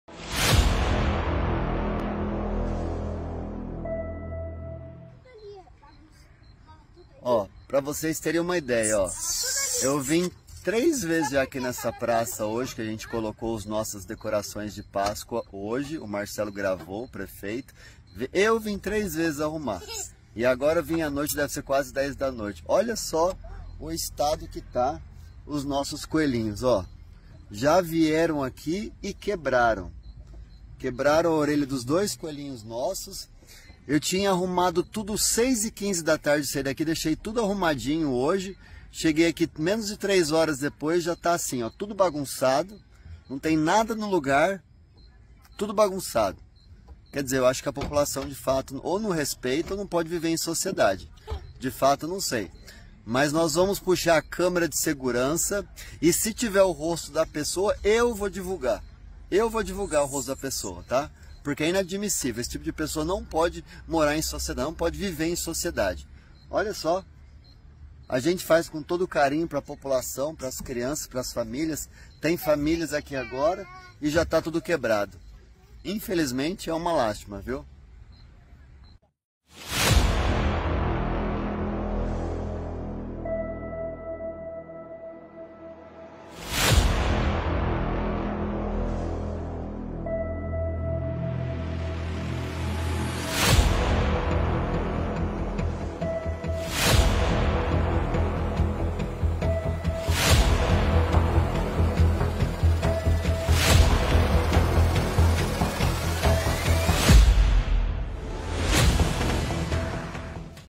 Decoração de Páscoa em Quatá é alvo de vandalismo horas após ser instalada
A decoração de páscoa da cidade de Quatá foi alvo de vandalismo no último domingo. As peças já foram restauradas mas a prefeitura vai investigar o caso para identificar os responsáveis.